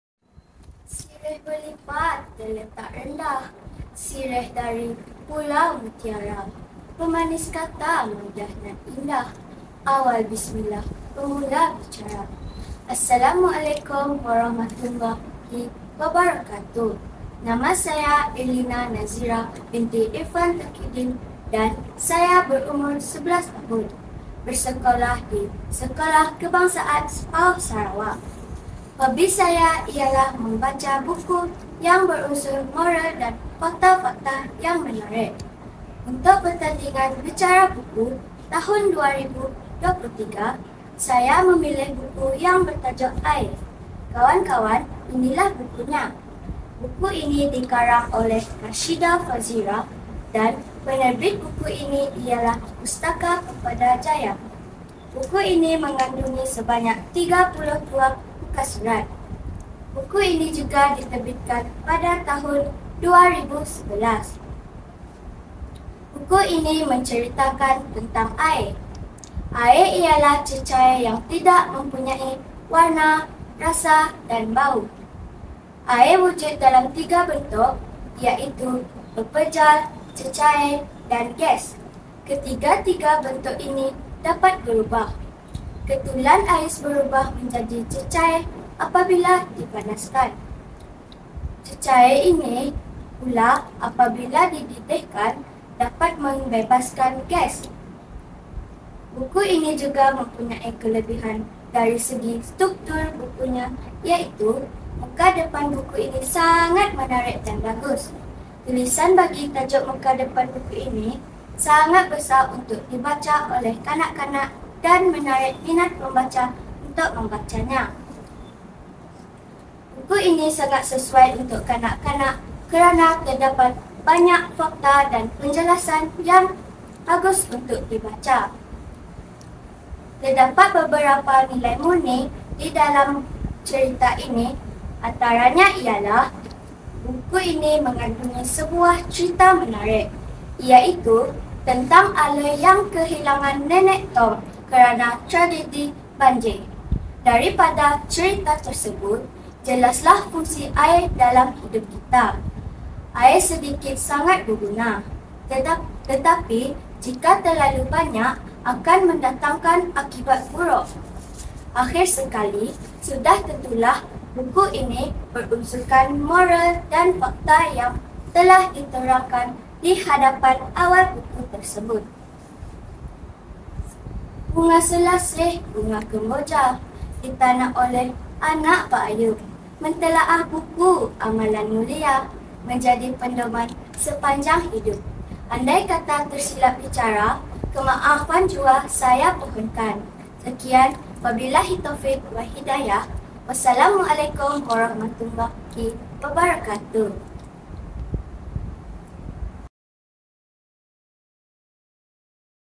SK SPAOH: PERTANDINGAN BICARA BUKU PPD BETONG 2023 - KATEGORI BAHASA MELAYU SEKOLAH RENDAH
Peserta: IRLINA NAZIRA BINTI IRFAN TAQIUDDIN